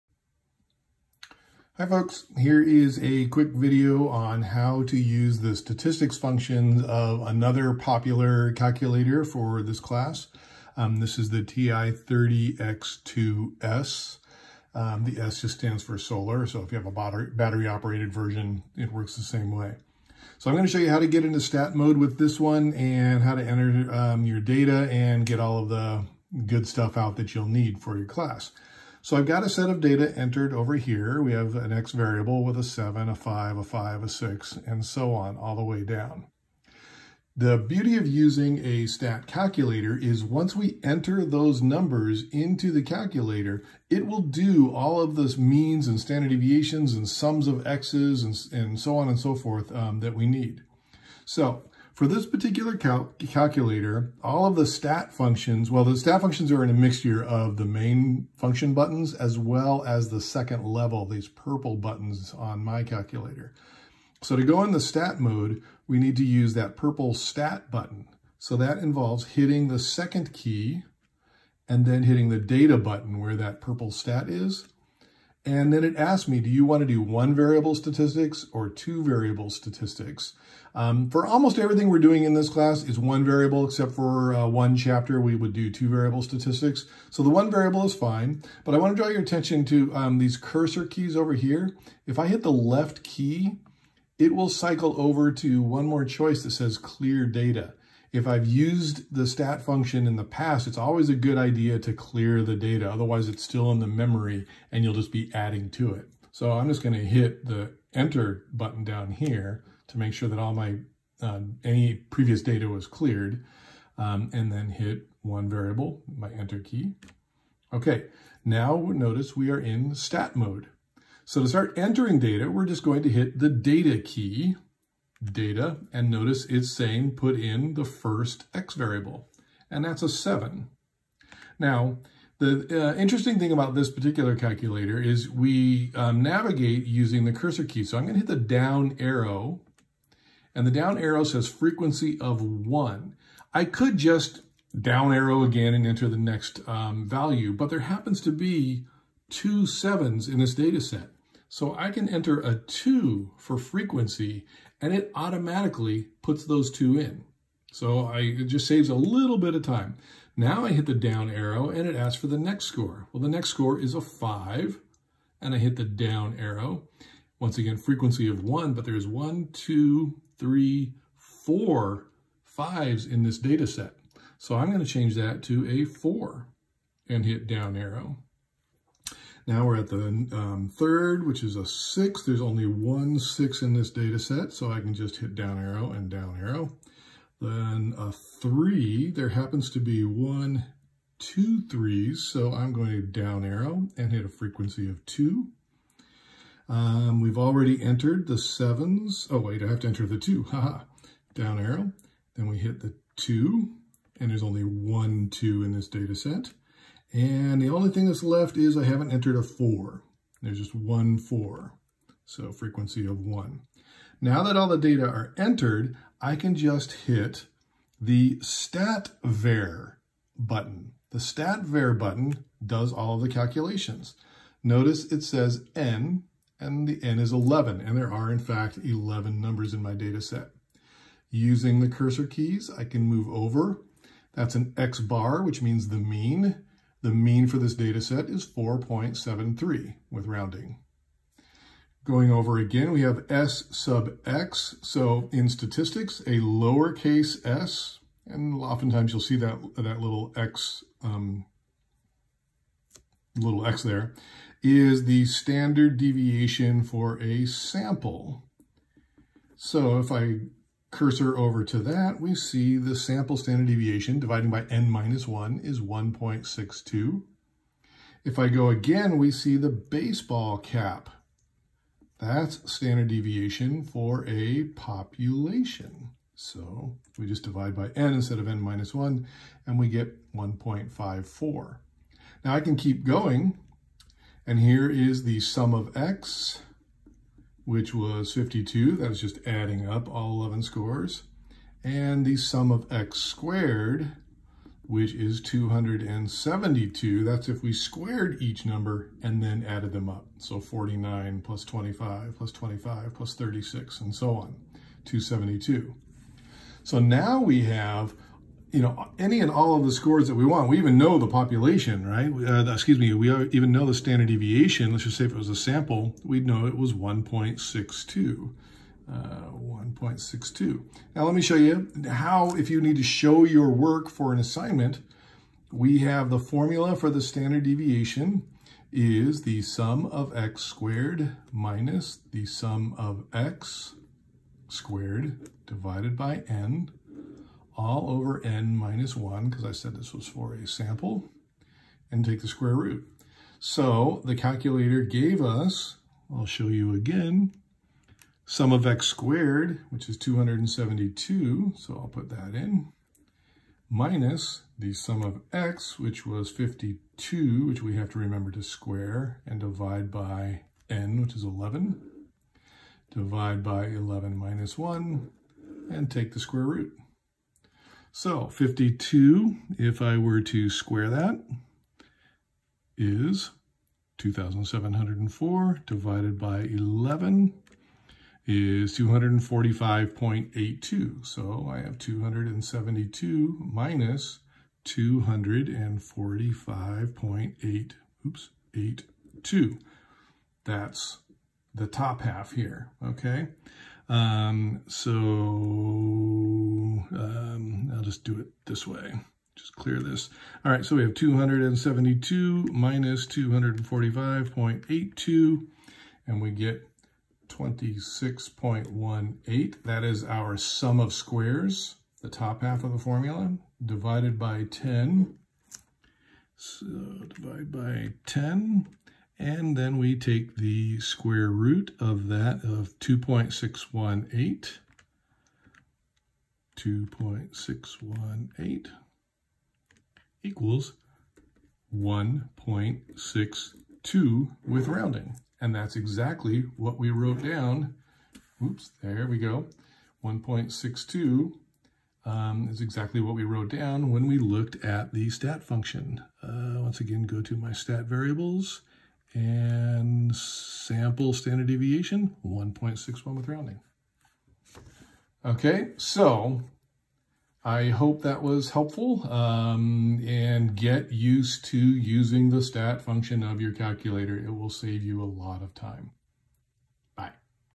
The TI-30XIIS calculator for statistics
A brief tutorial on entering data and using the stat functions of the TI-30XIIS calculator.